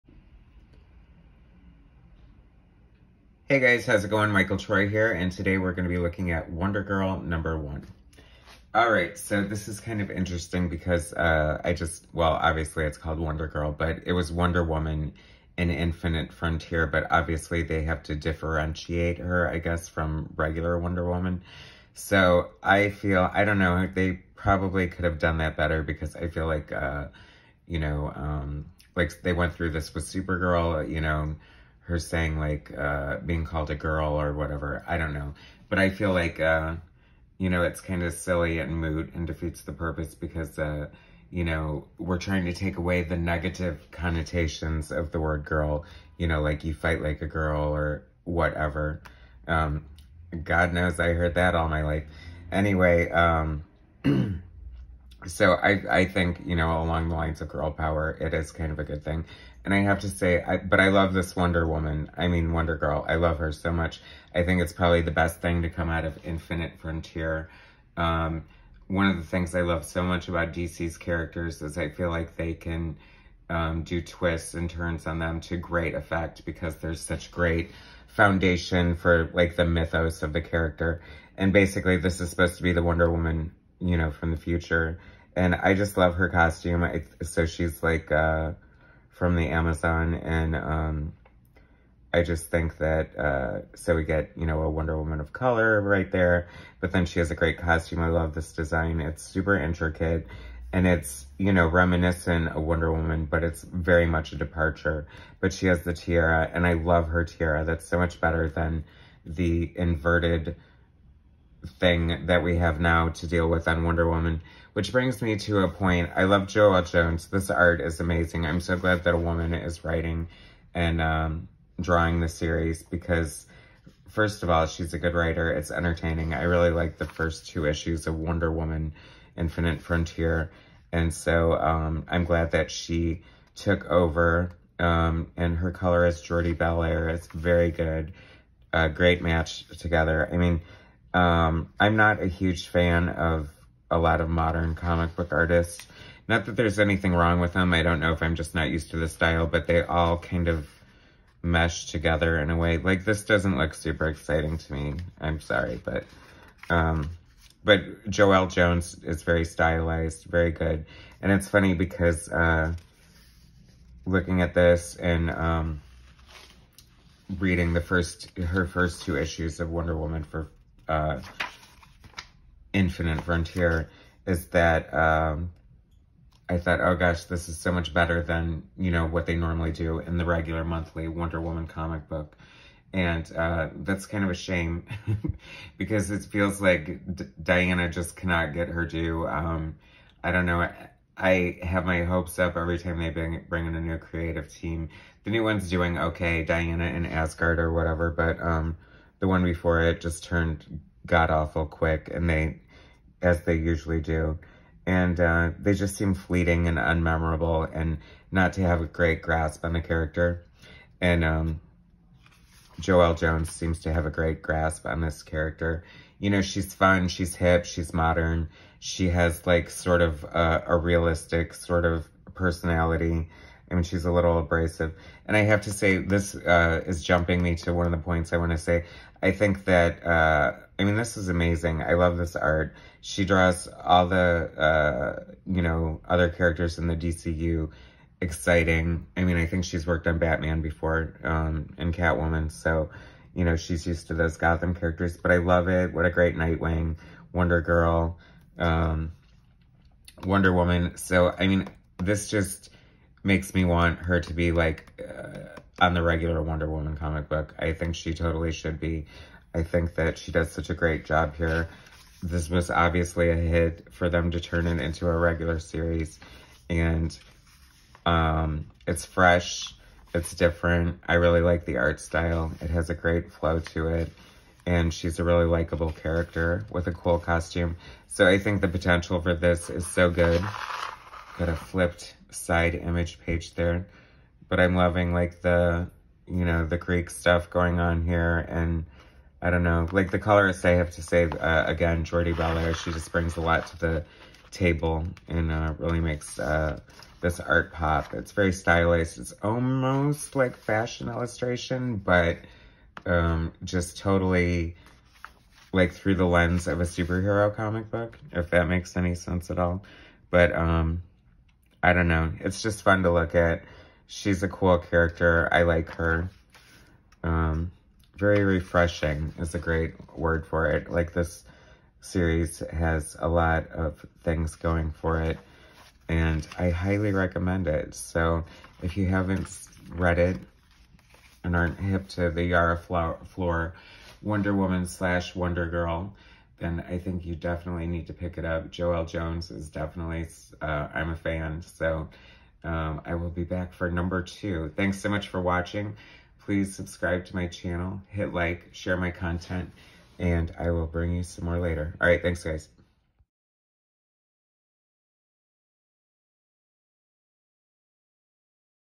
I Love Wonder Girl by Joëlle Jones
I think Yara Flor is one of the best things to come out of DC Comics Infinite Frontier event. Wonder Girl is one of the most realized new characters with a really cool costume that I have seen in a long time. Writer/Artist Joëlle Jones is perfect for the book. Please join me for a look inside.